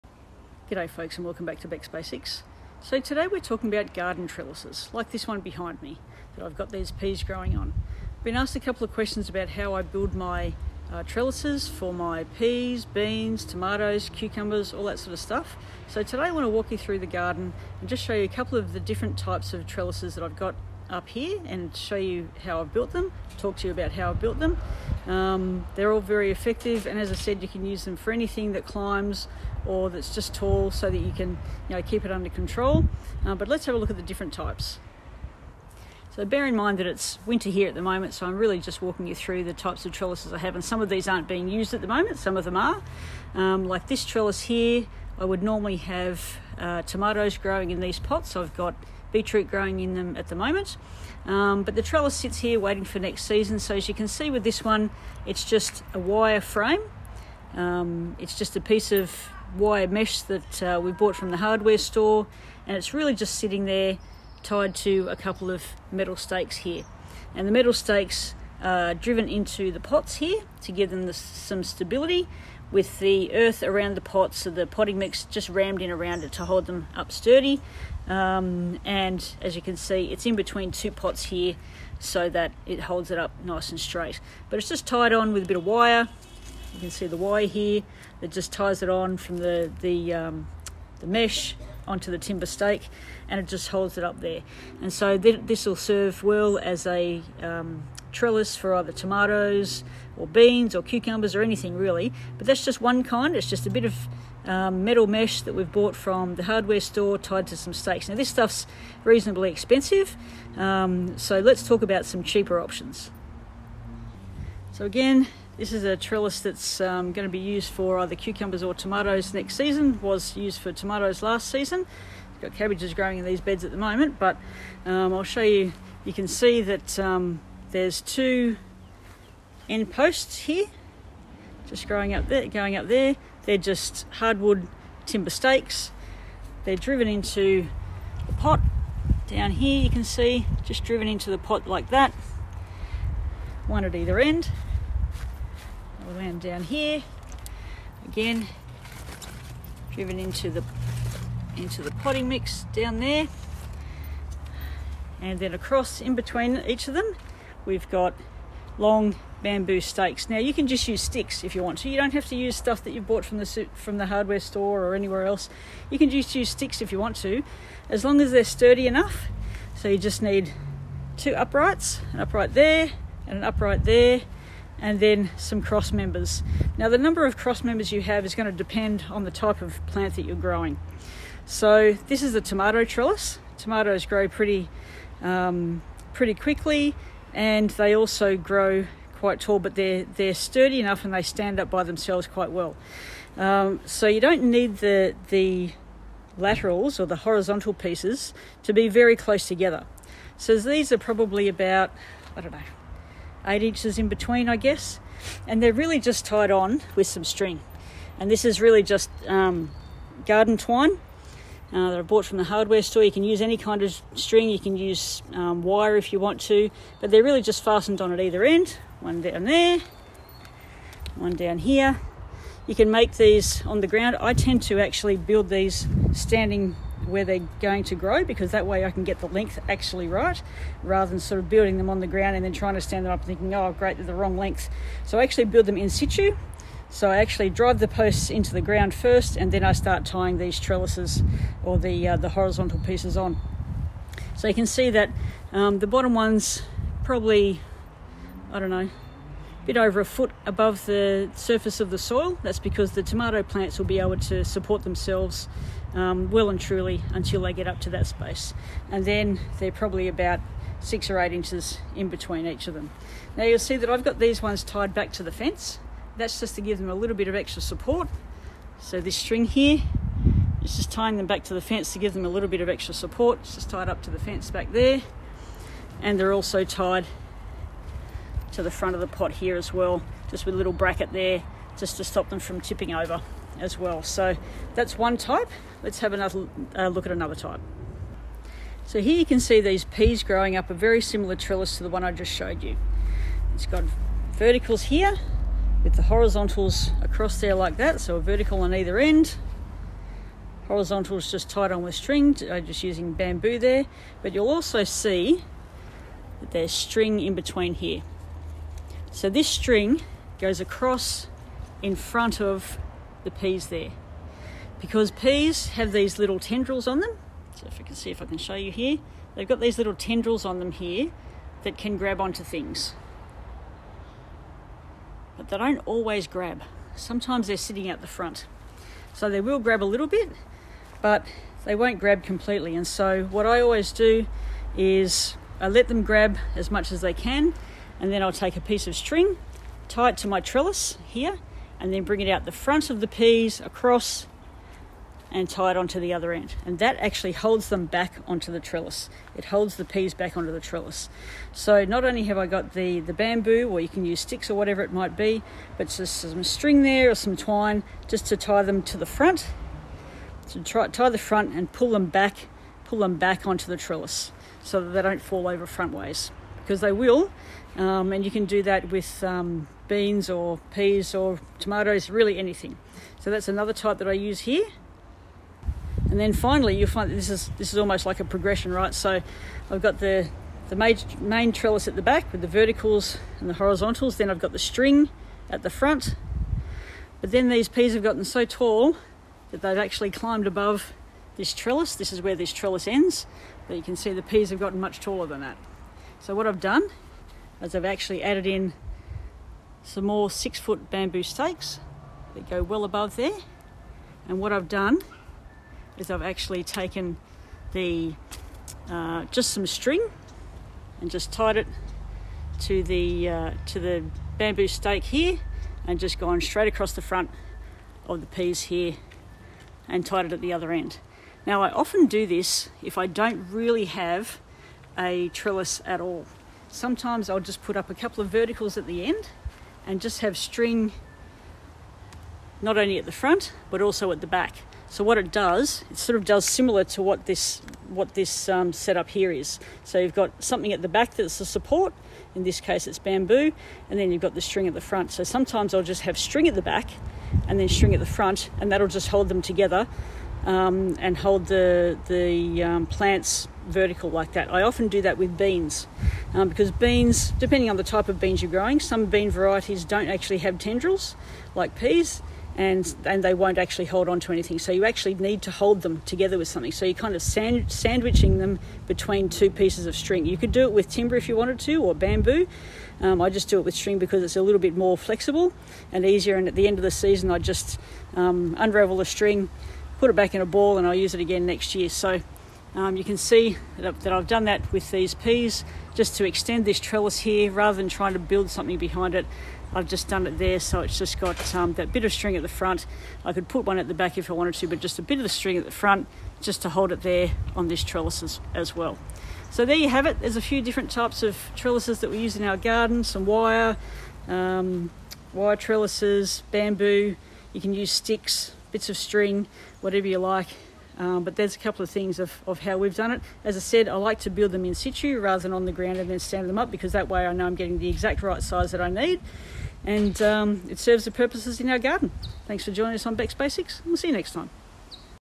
How to build a CHEAP and EASY DIY trellis for tomatoes, cucumbers, beans, squash, peas, etc
5 simple designs and that build on each other. Supporting your vertical plants especially in small gardens